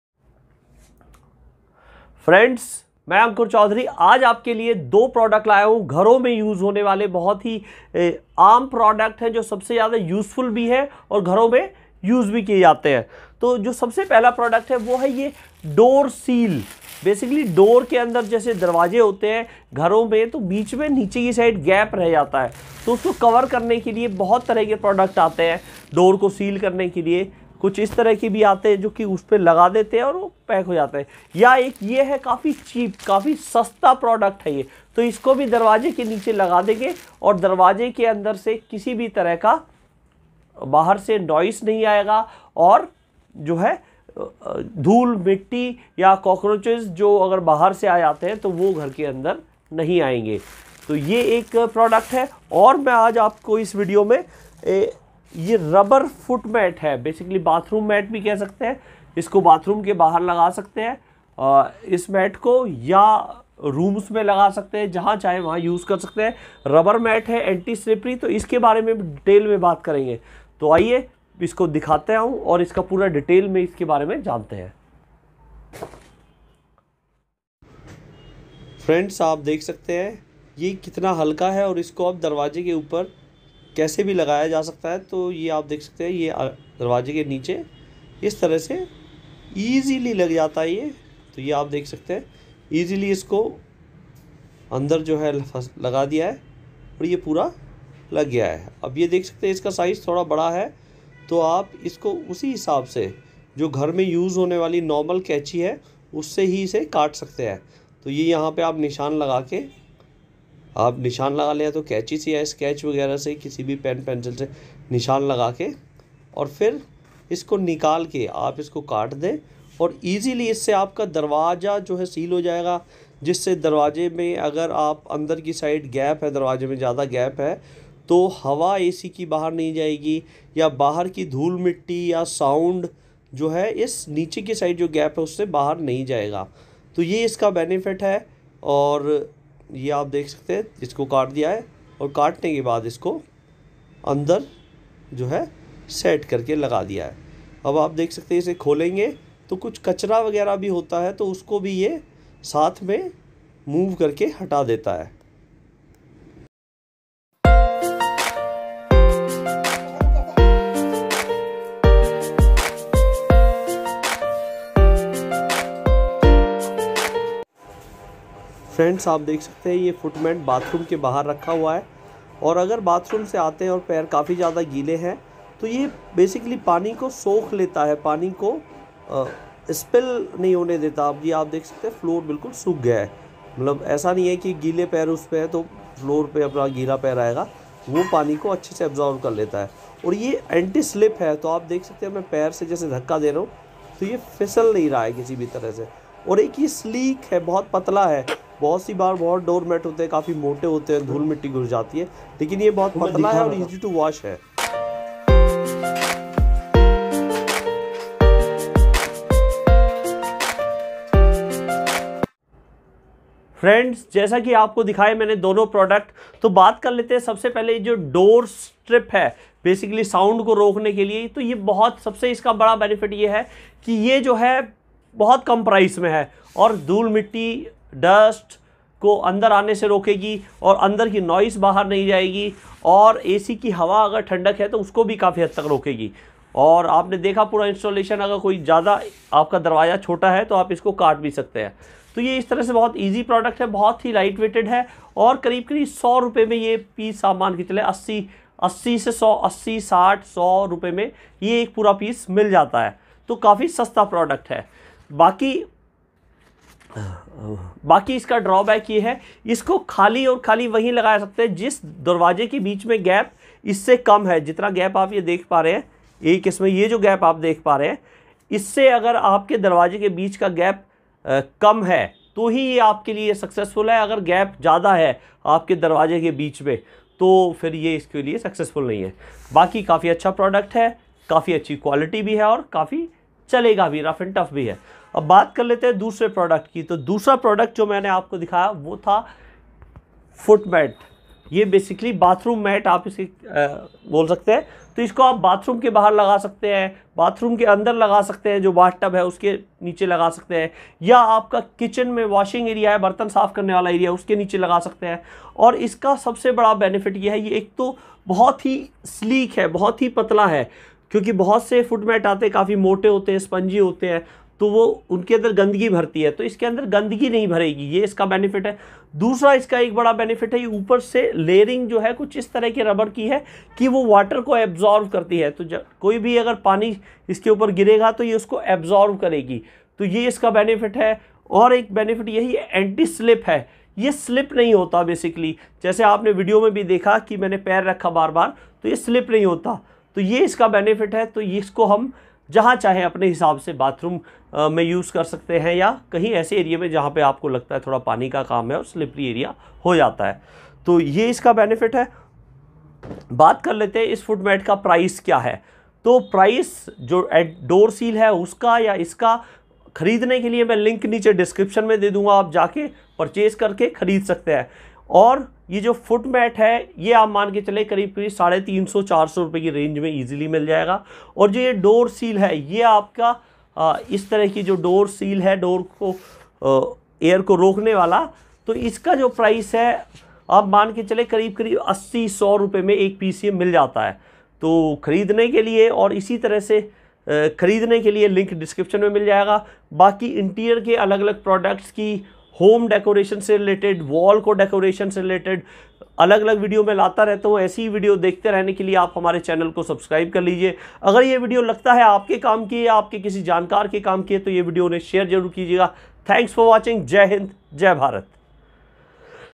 Bathroom mat & Door gape protector Information
In this video I Ankur Choudhary describe and provide information about Bathroom mat & Door gape protector
1) Door Bottom Sealing Strip Guard
Door Bottom Sealing Strip Guard for Home 36 Inch Under Door Draft Stopper is a double sided insulating device for doors and windows the fabric draft stopper sleeve holds foam cylinders on opposite sides of a door, creating an airtight, draft-proof seal.
Easily removable draft door blocker is easy to fix to the door. Made of high quality vinyl, durable and easy to clean. Just use cloth to wipe ,no need put in water or machine washing.
2) Bathroom Mat
INNOVATIVE ABSORBENT BATH MAT- Advanced Nano technology and up to 90% porosity surface allows water penetrates fast. The water absorption is up to 2-4 times of its own weight
QUICK DRY BATH MAT- Numerous tiny pores within the bathroom floor mat allows water evaporates quickly in the natural air. Usually, watermarks can magically vanish within few minutes. It won’t be damp when next one step on
ANTI-SLIP BACKING & COZY SURFACE-Rubber backing makes your bath mats for bathroom non slip even on wet floor. Innovative leather-like surface looks like diatomaceous earth bath mat but feels like Chamois leather, you won’t feel cold when you stepping on the bath rugs for bathroom in cold weather
DURABLE & NO SHEDDING- No microfibers, therefore you will never meet troubles caused by traditional microfiber bath mat like pilling, fading, shedding, and out of shape. Furthermore, the rubber bathmat won’t be smelly after being wet for long time
DIRT RESISTANT & EASY TO CLEAN- No need to wash. Smooth surface designed to resist dirt and hair. Just wipe the rugs for bathroom with wet cloth or rinse with shower nozzle can make your shower rug as well as new

(door gape protector)
(Door Bottom Sealing Strip Guard)